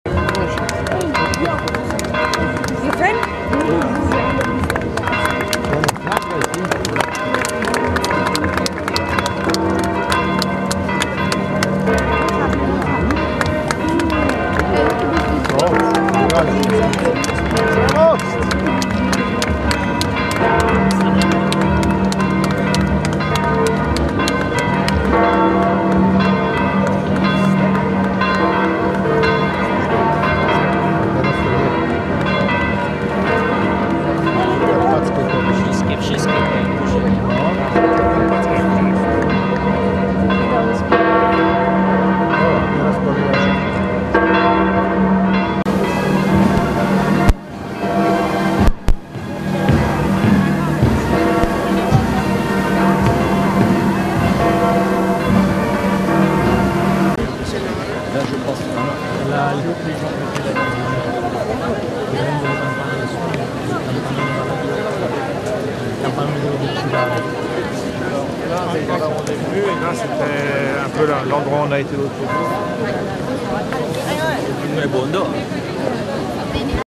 Il suono delle campane di San Pietro nel giorno di Natale
(Agenzia Vista) A seguito della benedizione Urbi et Orbi di Papa Francesco, le campane di San Pietro hanno suonato per salutare il Natale. Per l'occasione speciale ha suonato anche il campanone, detto Valadier, del peso di 9 tonnellate e con un diametro di due metri.